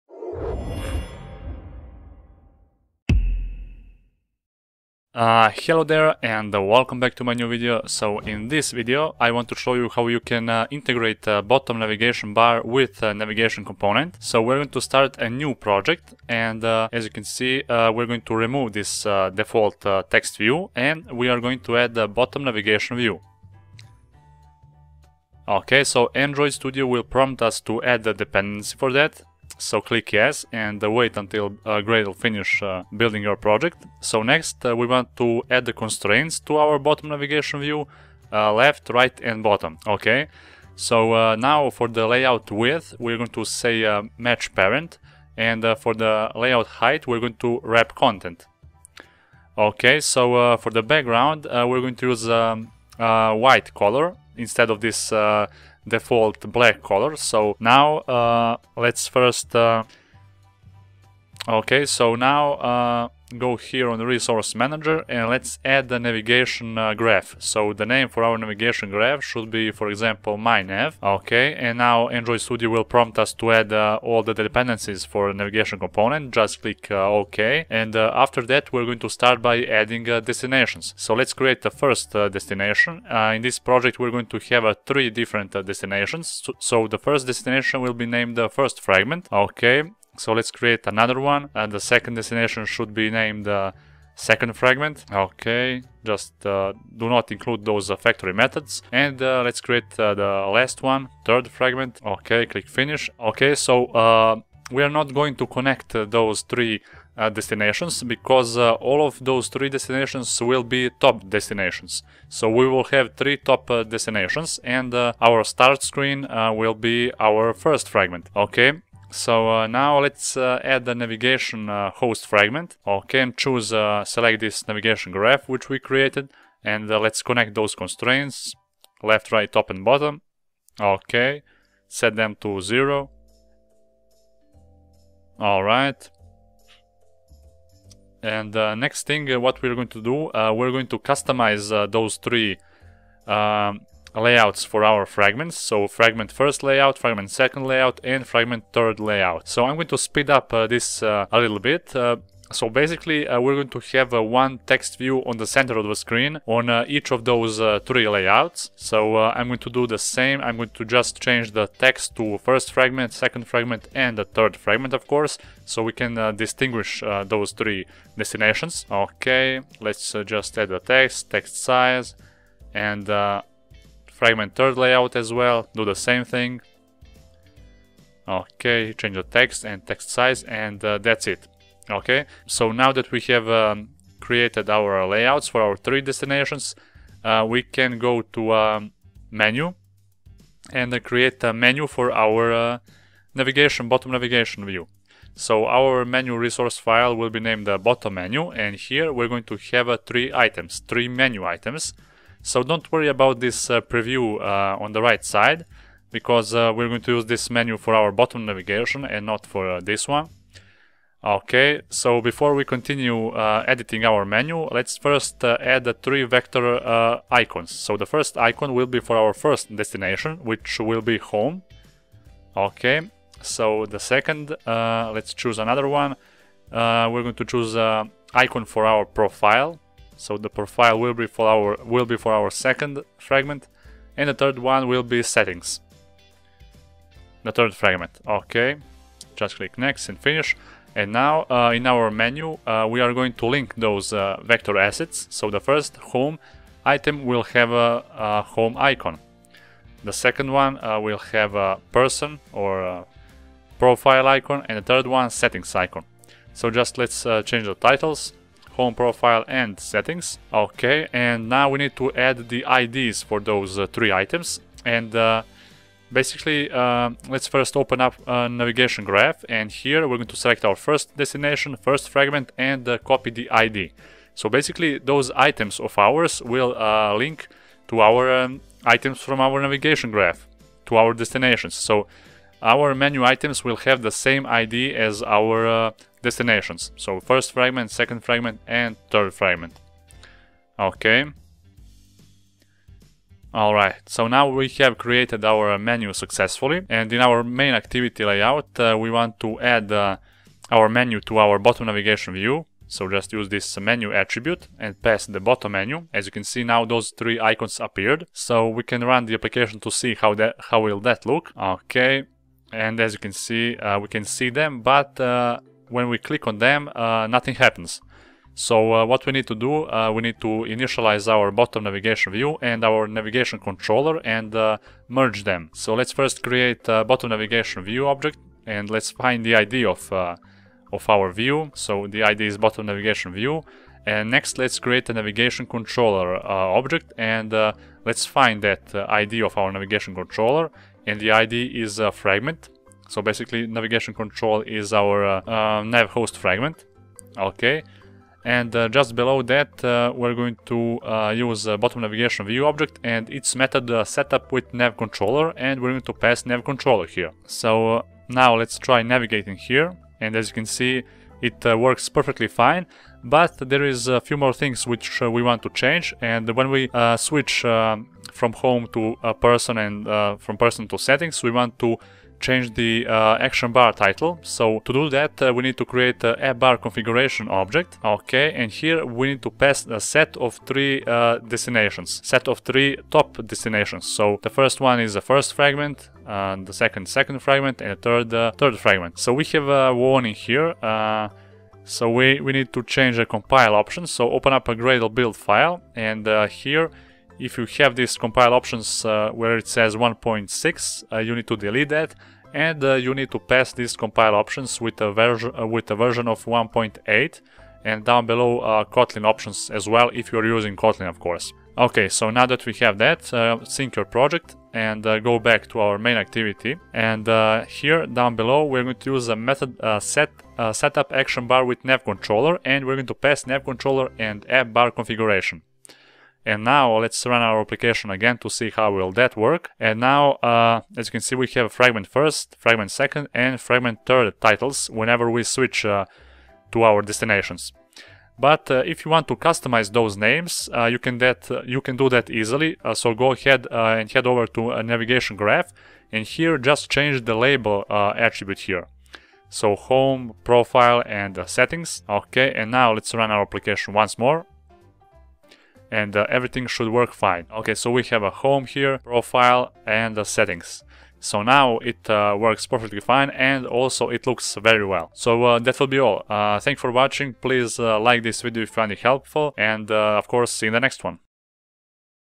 BottomNavigationView with Navigation Component - Android Studio Tutorial
Bottom Navigation View is a material component that makes it easy to explore and switch between the top-level destinations in single click or tap.The bar contents can be populated by specifying a menu resource file. Each menu item title, icon and enabled state will be used for displaying bottom navigation bar items.

*** Compile Options ***
compileOptions {
        sourceCompatibility JavaVersion.VERSION_1_8
        targetCompatibility JavaVersion.VERSION_1_8

    kotlinOptions {
        jvmTarget = JavaVersion.VERSION_1_8.toString()